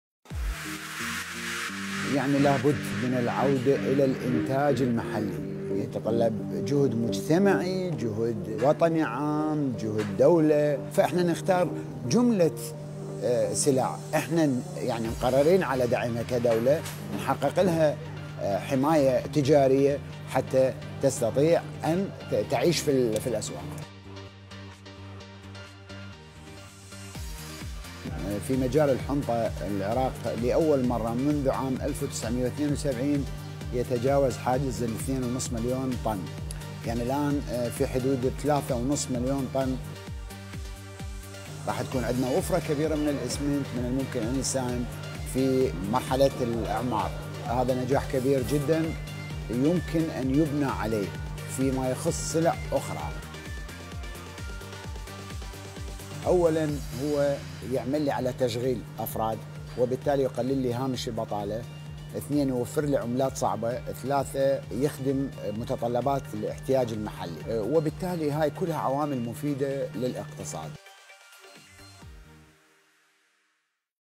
اهمية المنتج المحلي
رأي خبير اقتصادي عراقي في أهمية الإنتاج المحلي وتطوره في السنوات الأخيرة. هل للمواطن دور في دعم اقتصاد بلده؟ وكيف؟